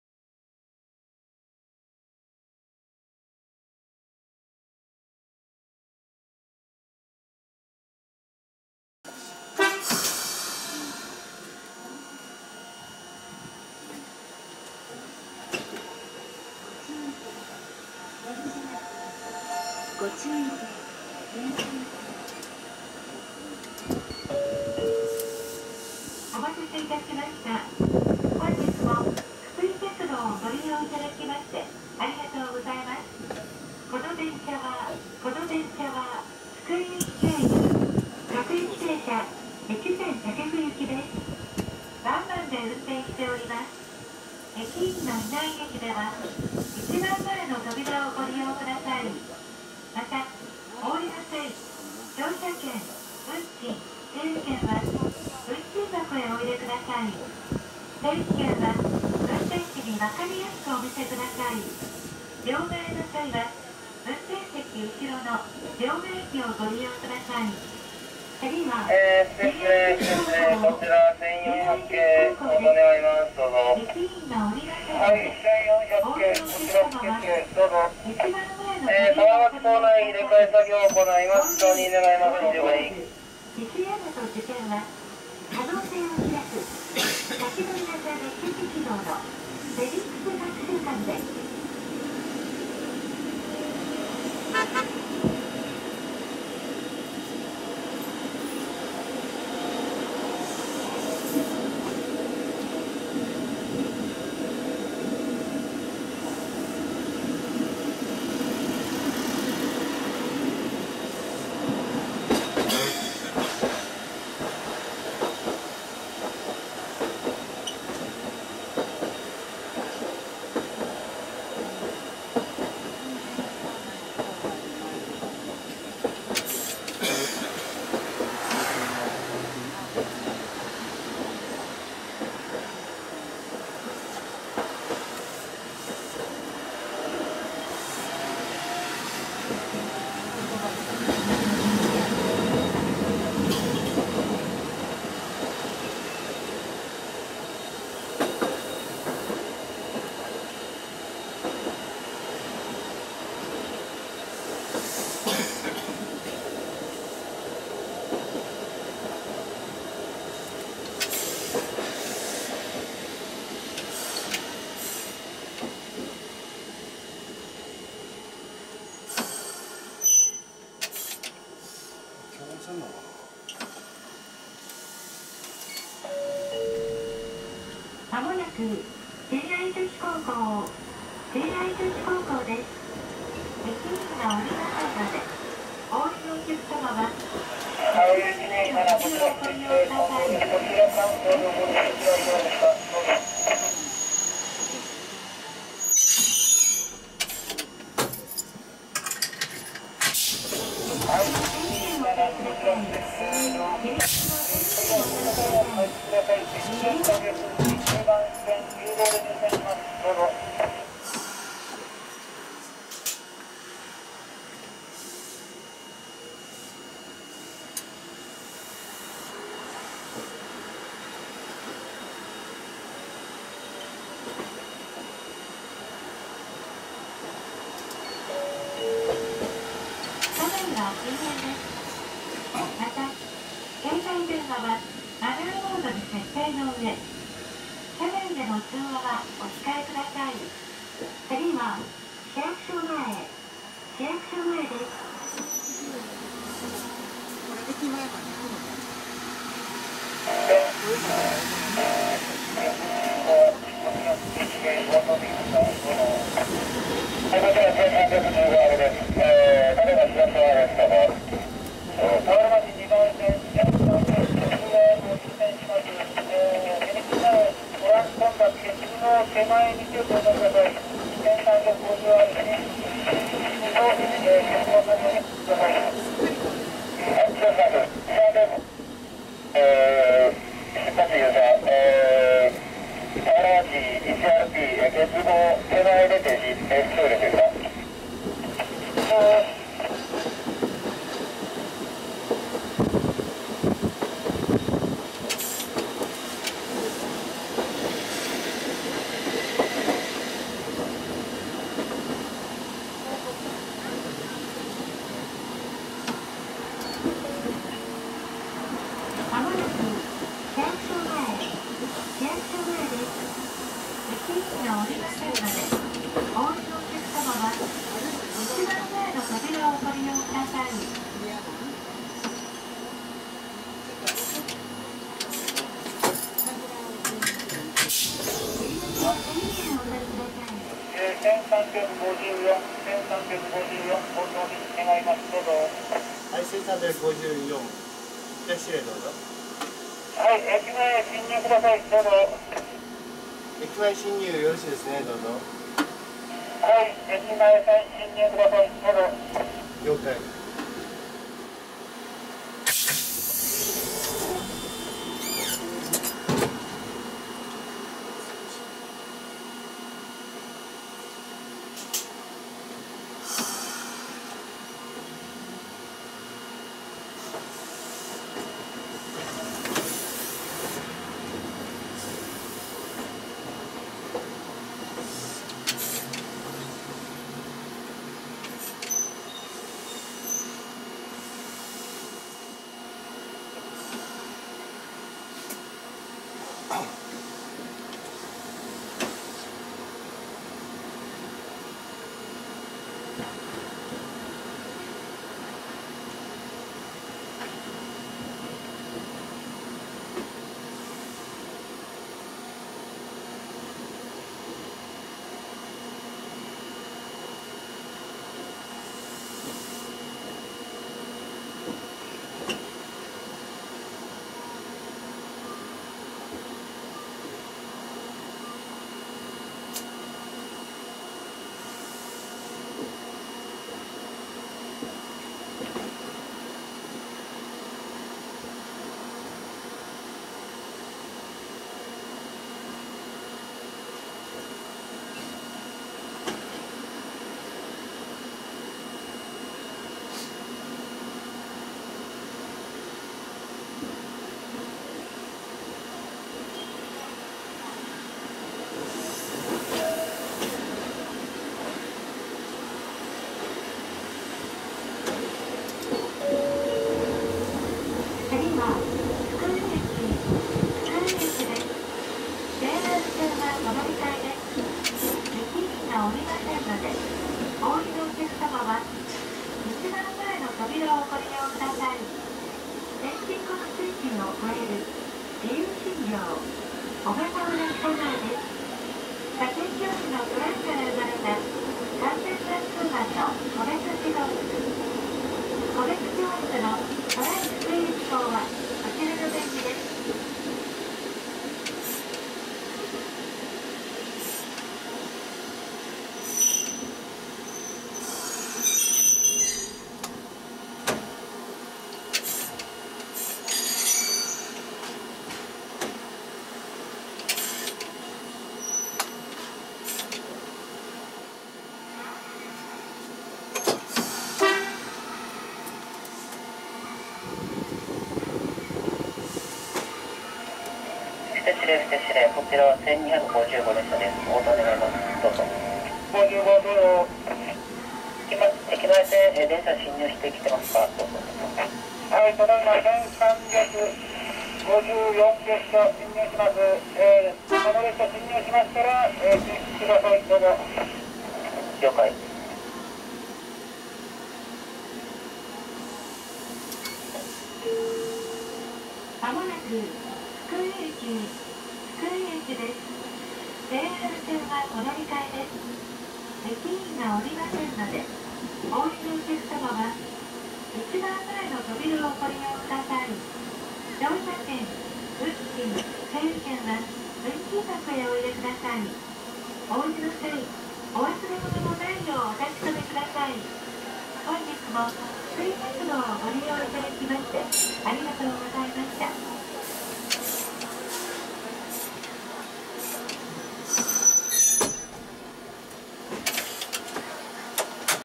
福井鉄道駅前線 駅前広場延伸 前面展望 田原町-福井駅
【手持ち撮影のため、手ブレ等はご容赦願います】
福井鉄道福武線の一部区間として、市役所前から福井駅方面へ分岐する枝線、通称駅前線。
従来、ユアーズホテルフクイ近くの福井駅前停留所が終点でしたが、2016年3月27日に駅前広場まで約150m延伸し、福井駅停留所が新設されました。
田原町から福井駅までの、770形の前面展望です。
Cab view of Fukui Railway from Tawaramachi to Fukui Station.